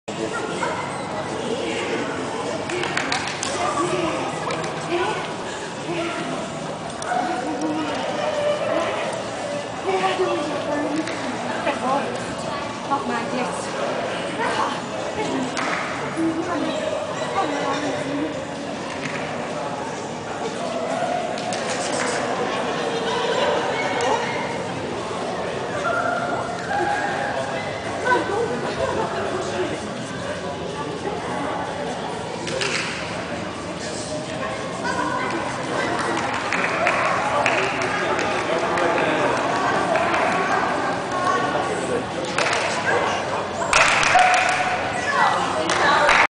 doggy-agility 26
Agility Kaiseraugst, Graber Conny mit Tyson - Jumping Small 1